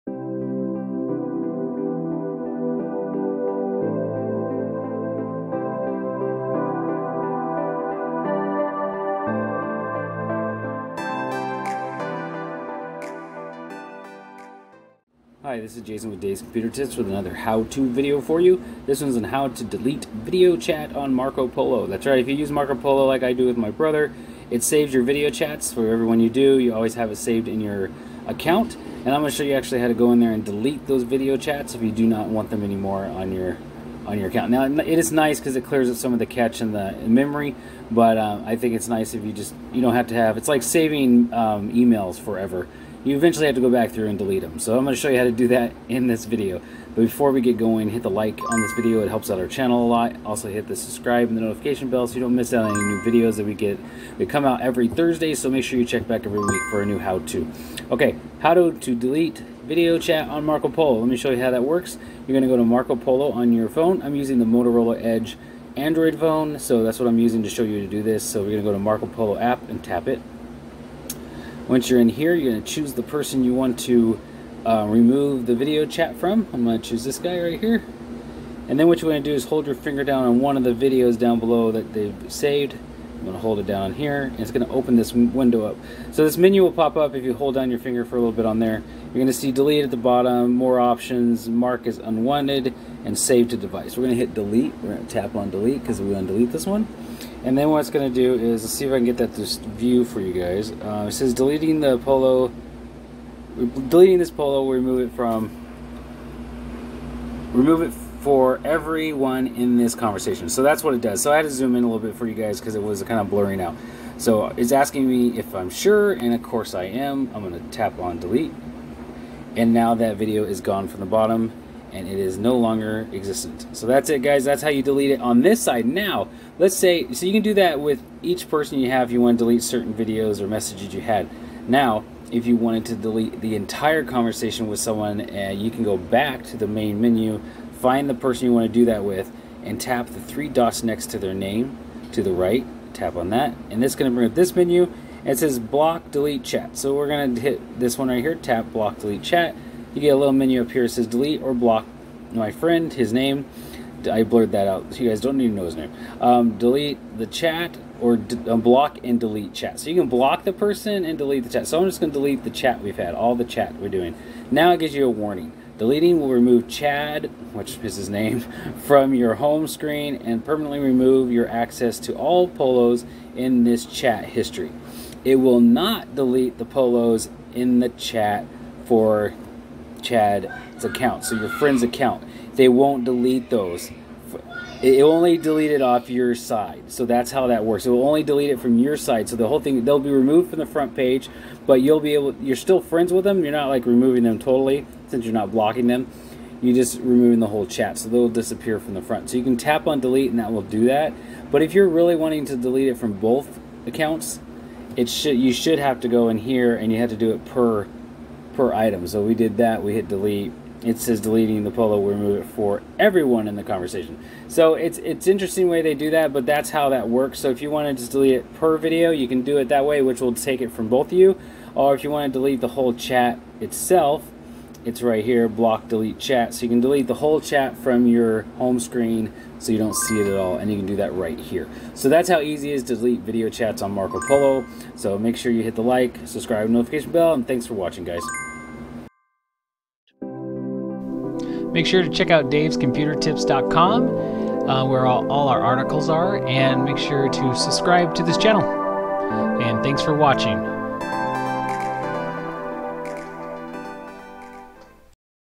How To Delete Video Chat On Marco Polo
In this video, I will show how to delete the video chats off your Marco Polo.

I spend a lot of time using an app called Marco Polo, a great app to send video messages to family and friends. My brother and I use this every day since he lives about three hours away from me. The reason I wanted to bring this up is that I had a question sent to me from a DCT viewer. Not sure if it came from my article or my videos but either way, this is what they asked, “How do you delete the Video chat messages all at once in my Marco Polo app?” This is something I never thought about but now that I have, I wanted to share my knowledge with all of you. Before we start, this lesson is another video I have created about this app:

Now that you are caught up, let’s add another lesson to this. Time to pull out those devices and get this started.